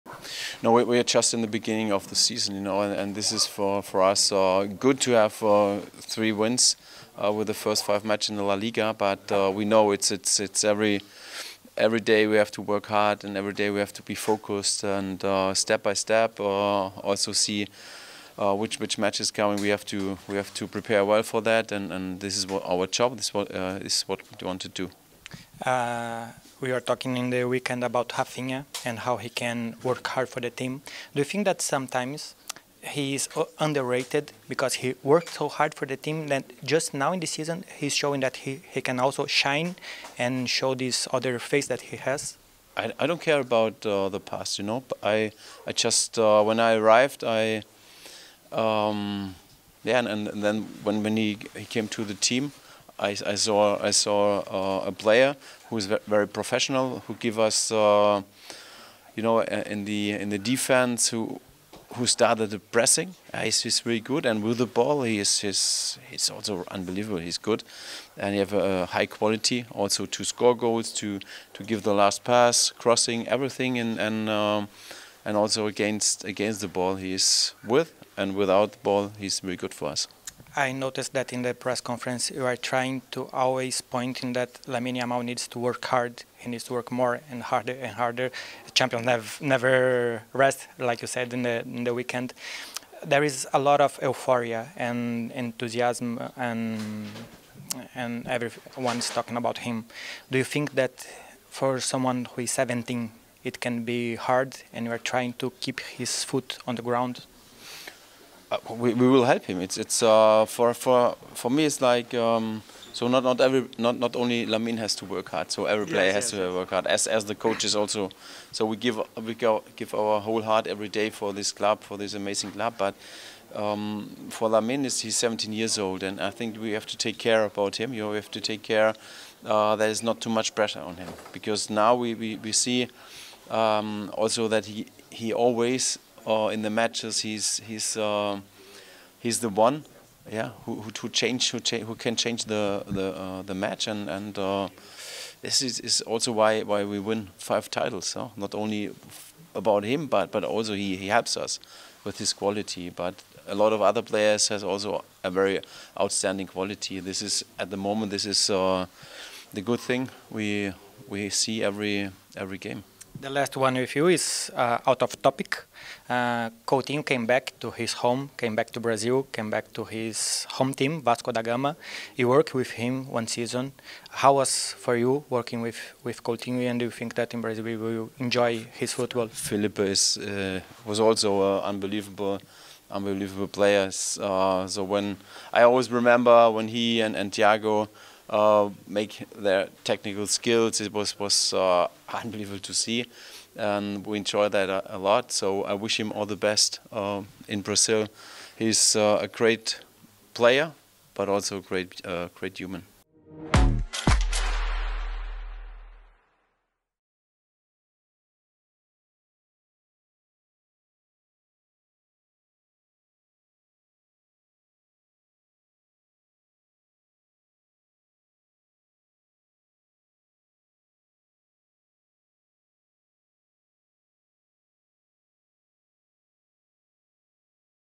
HANSI FLICK: "LAMINE YAMAL PUEDE CAMBIAR PARTIDOS" | ENTREVISTA
Hansi Flick, técnico del FC Barcelona, destacó la confianza que tiene en la joya Blaugrana, Lamine Yamal, quien consideró que es el indicado del club para cambiar el rumbo de los partidos.

Pese a ello, previo a hacer frente a la Jornada 1 de la UEFA Champions League 2024/25, Flick quitó presión para Lamine, pues consideró que su equipo tiene más individualidades para marcar la diferencia dentro de la cancha.

Además, Flick también se dio el tiempo para elogiar las cualidades de Raphinha y un viejo conocido: Philippe Coutinho.

No te pierdas los partidos de la UEFA Champions League, la Premier League y la Emirates FA Cup por Max